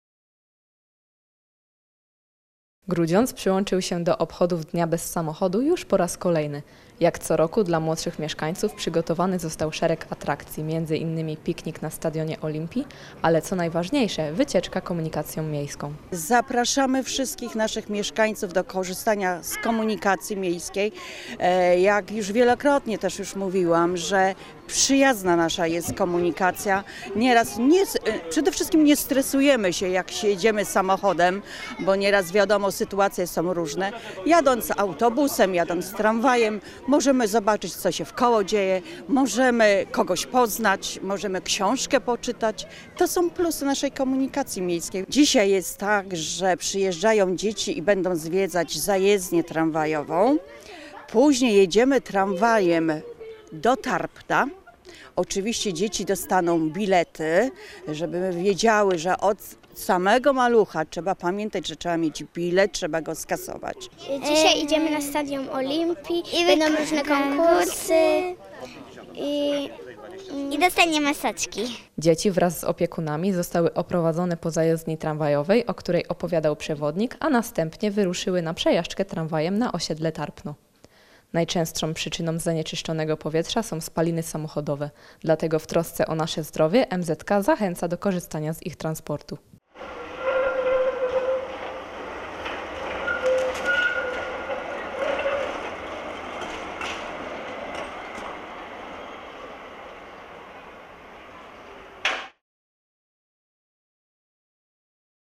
26-09-2016 Dzień bez samochodu
Grudziądz przyłączył się do obchodów dnia bez samochodu już po raz kolejny..